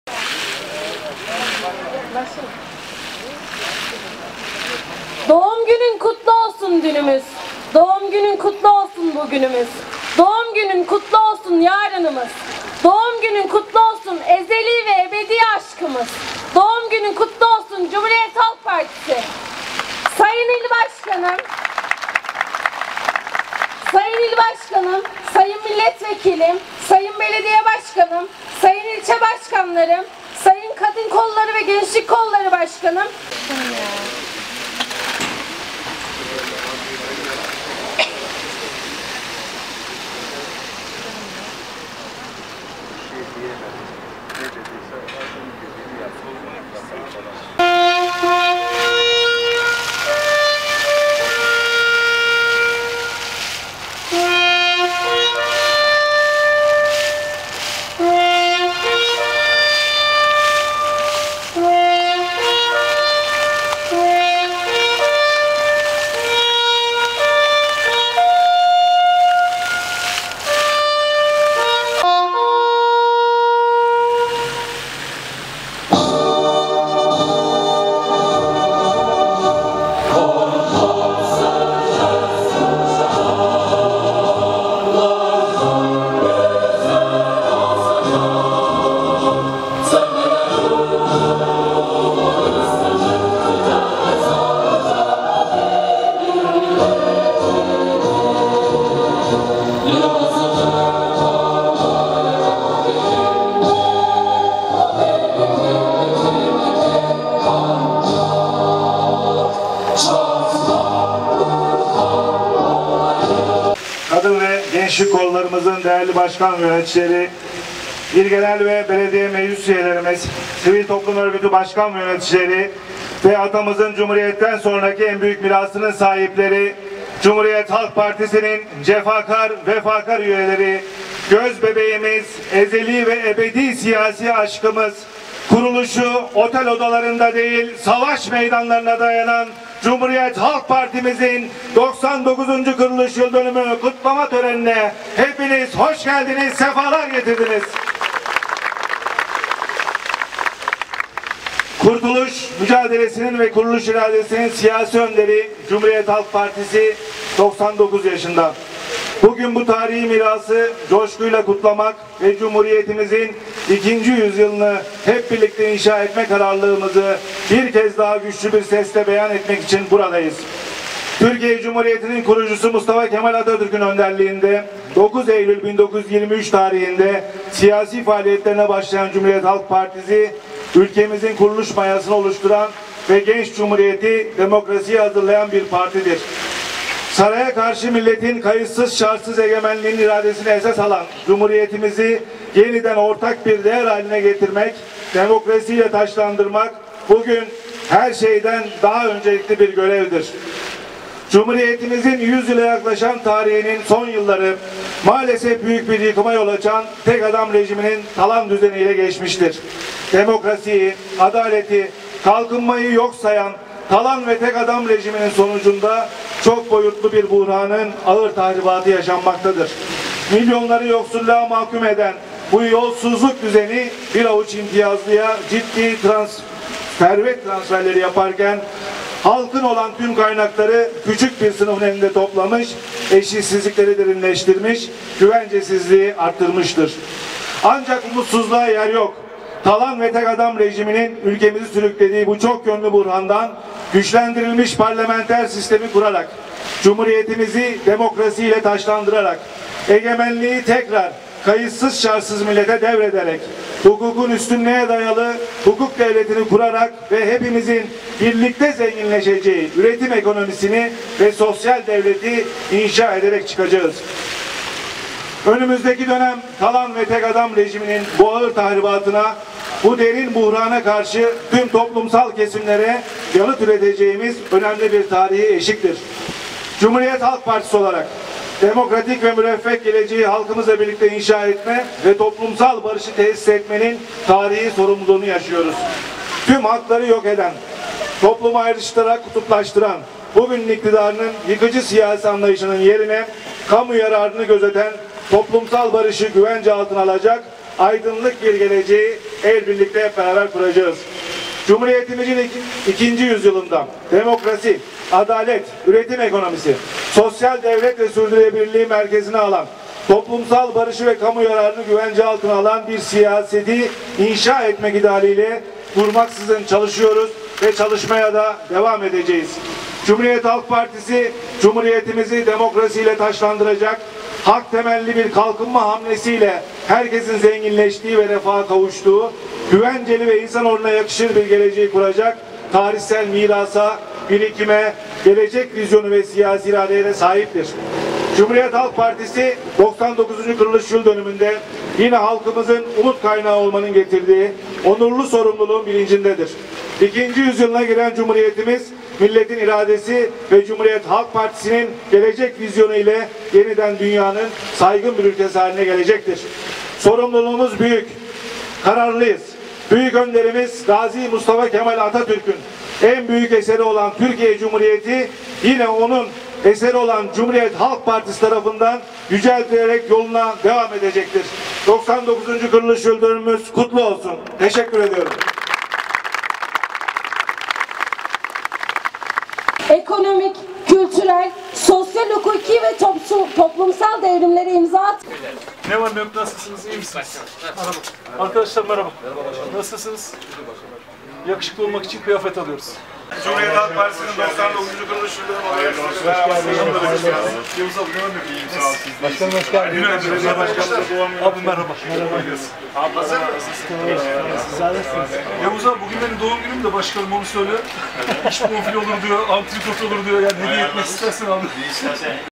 CHP'nin 99 kuruluş yıldönümü-2022
CHP, 99’uncu kuruluş yılını kutladı
Kurtuluşun ve kuruluşun partisi, Gazi Mustafa Kemal Atatürk'ün emaneti Cumhuriyet Halk Partisi kuruluşunun 99’uncu yıldönümünü kutluyor. CHP Burdur İl Örgütü, kutlama programı çerçevesinde Atatürk'ün manevi huzuruna çıkarak çelenk sundu. CHP Burdur İl Başkanı İzzet Akbulut törendeki konuşmasında, CHP'nin 2'nci yüzyılda da cumhuriyeti demokrasiyle taçlandıracağını belirterek, “Saraya karşı milletin kayıtsız şartsız egemenliğinin iradesini esas alan Cumhuriyetimizi yeniden ortak bir değer haline getirmek, demokrasi ile taçlandırmak bugün her şeyden daha öncelikli bir görevdir. Cumhuriyetimizin yüz yıla yaklaşan tarihinin son yılları maalesef büyük bir yıkıma yol açan Tek Adam Rejiminin talan düzeni ile geçmiştir” dedi.
Alev Coşkun’un da hatırlattığı gibi, her ne kadar Cumhuriyet Halk Partisi (CHP’nin) kuruluş tarihi, fiilen 4 Eylül 1919 Sivas Kongresi’ne dayansa da sonradan düşmanın denize döküldüğü tarih olan 9 Eylül esas alındığı için 99’uncu kuruluş yıldönümü kutlanıyor.
Burdur’da da CHP İl Örgütü’nce büyük Atatürk’ün manevi huzurunda çelenk sunuldu.
Akşamüzeri de parti binaları önünde pasta kesilip kokteyl verilecek.
Gününü anlam ve önemi üzerine İl Başkanı İzzet Akbulut bir konuşma yaptı.
Konuşmayı müteakiben Gazi Caddesi üzerindeki esnaflara ziyarette bulunuldu.
Akbulut, konuşmasında şu ifadelere yer verdi.